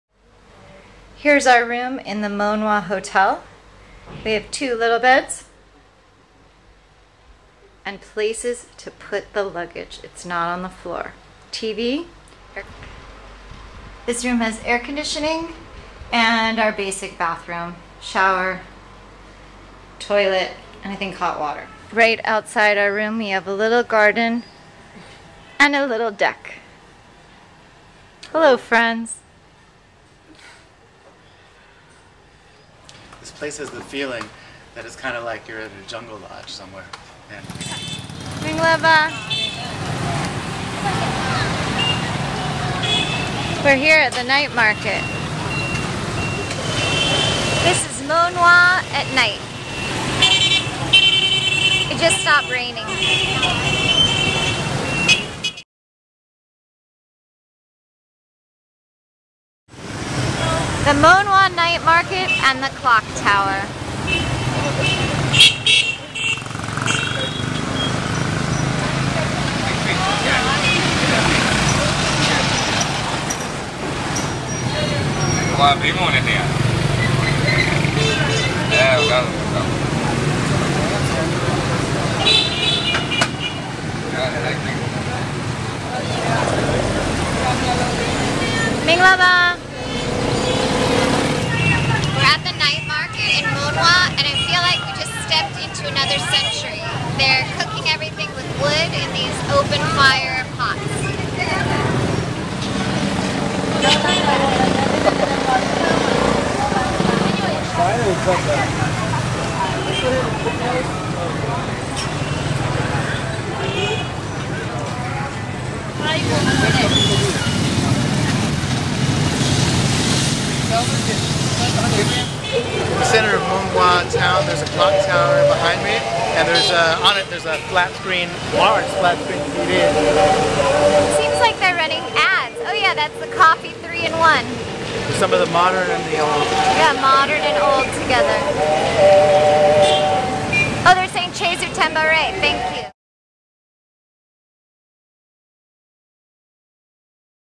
19 Monywa Arrival and Night Market, Myanmar (Burma)
We Said Go Travel See video 18 for the 12 hour journey from Hsipaw to Monywa. This video starts with our arrival in Monywa.

After our long journey, we needed to find a place to stay. Unfortunately the Shwe Taung Tarn has definitely seen better days and even for $15 with breakfast I said no thanks. The fourth floor rooms (after passing the grime, dirty trashcans and broken windows) had three twin beds ala the Seven Dwarfs, but the sheets had holes, the air conditioning unit was in pieces and I just did not think it was for us. The electricity would not be on until later in the day so we could not determine if it worked and there were no fans. The woman at the desk did have room 401 as well (we looked at 402 and 403). We asked about the newer building which was $22 but that was all full.

At first, the Monwya Hotel said we could only stay one night but George negotiated an early departure and they said YES to two nights. The hotel cost $30 with air con and breakfast and has fluffy white towels and hot water. I like the deck and the bright lights and George is happy to have a mini-fridge. There is wifi in the restaurant area which does not work great.

How much does a travel day cost?
Bus 4500 x 2
Xfer 6000 for 4: 3000
Bus 2000 x 2
Snacks 2300
Tuk tuk 2000 for 4: 1000
Room: $30
Dinner 1800 noodle soup/water: 4000
9000, 3000, 4000, 2300, 1000, 4000: 23300 + $30: $60ish for George and I. A day without so much transport is cheaper.

We enjoyed wandering in the Monywa Night Market.
October 7, 2012

This movie is from our 28 days in Myanmar (Burma) from September 28, 2012 to October 26, 2012 and our year TRIP in South East Asia, see all the videos from our trip